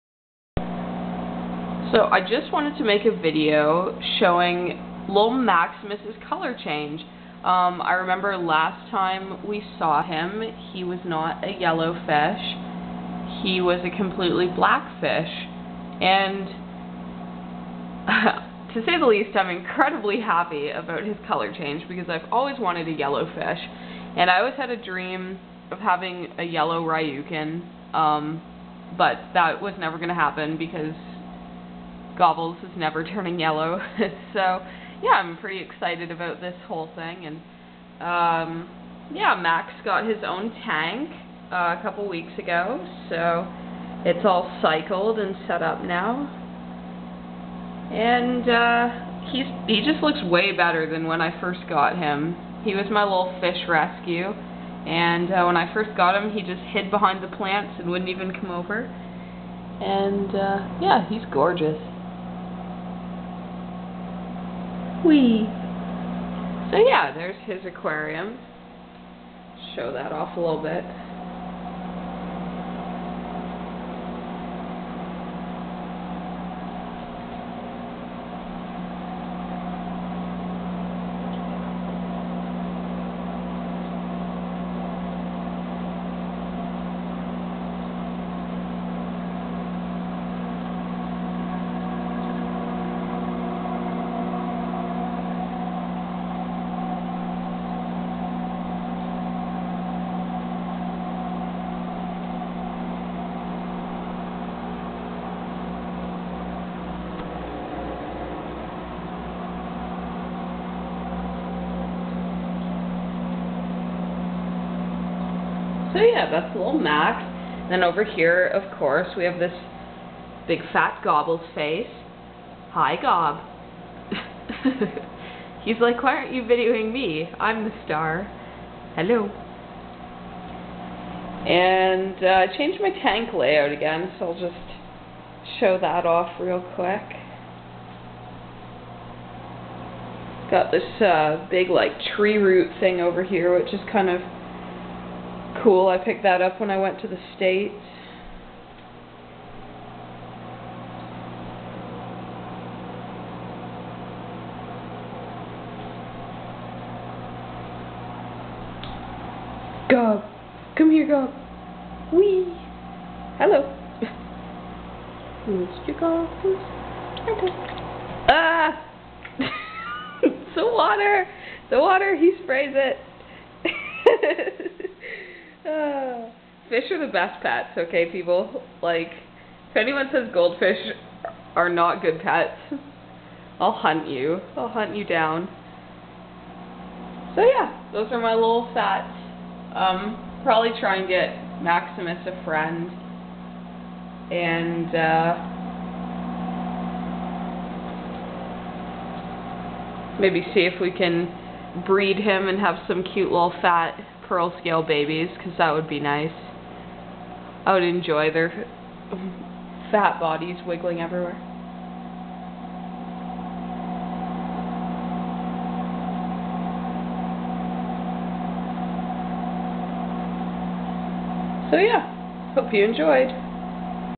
Gobbles and Maximus Livin' the Life
These are my two babies, Gobbles and Maximus. I'm sure everyone wonders why I have two aquariums, one a 75 Gallon and one a 30 Gallon, to house two goldfish. My answer is, because I feel like it. Don't judge my love for them.